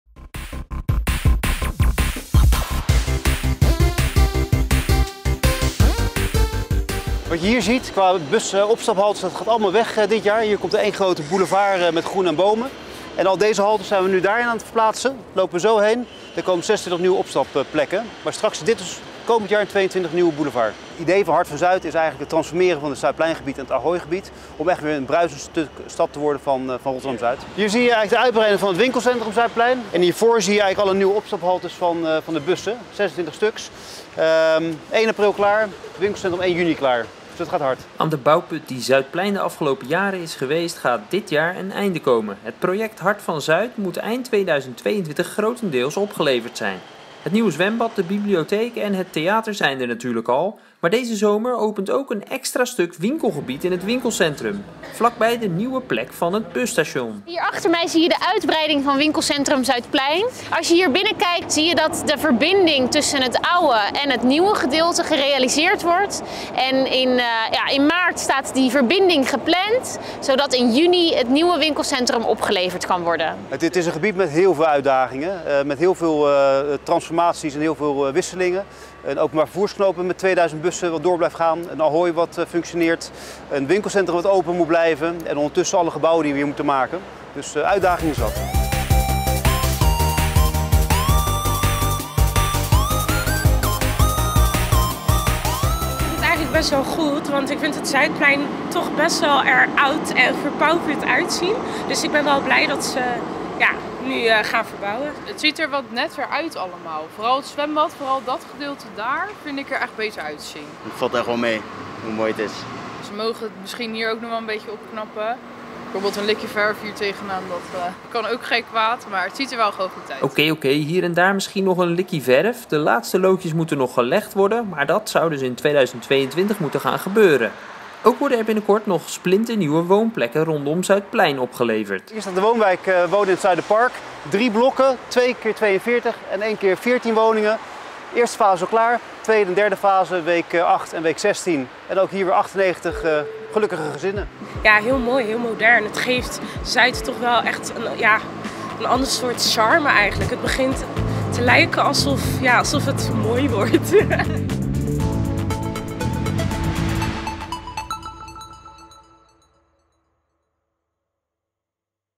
Ontwikkeling gebied Zuidplein bijna compleet: eind 2022 klaar | Trending
Hart van Zuid moet hét nieuwe centrum van Rotterdam Zuid worden. Een groot deel van het project staat er al, met bijvoorbeeld het nieuwe Theater Zuidplein, maar daarmee is het nog lang niet af. Het is de bedoeling dat het hele project eind 2022 wordt afgerond.